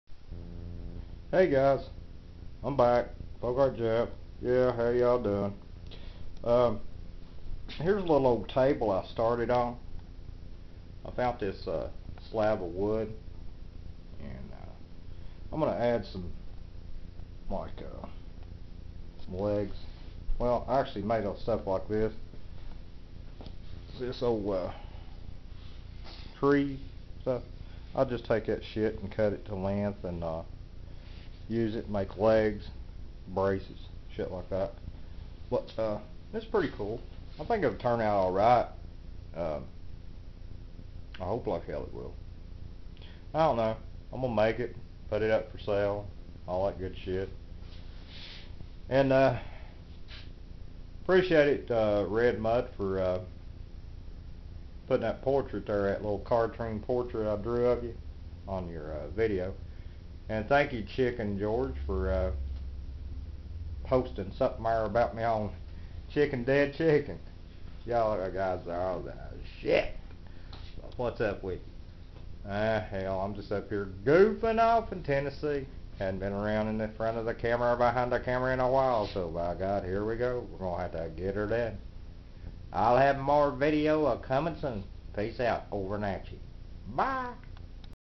" Folk Art Table " by ~ FolkArtJeff ~
Here is a " Rustic Hand made Table " that , I started to work on. More handmade Table's in the future ! Thank's ! ~ FolkArtJeff ~.